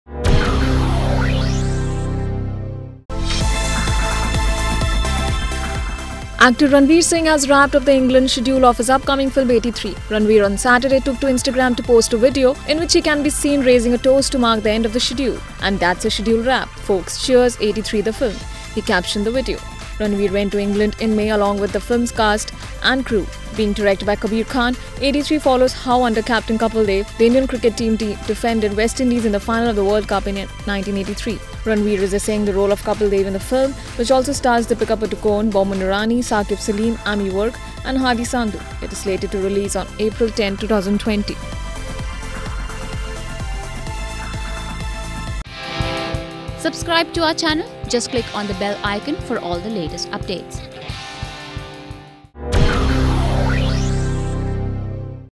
Ranveer Singh wraps up England schedule of '83'
Actor Ranveer Singh has wrapped up the England schedule of his upcoming film "'83'".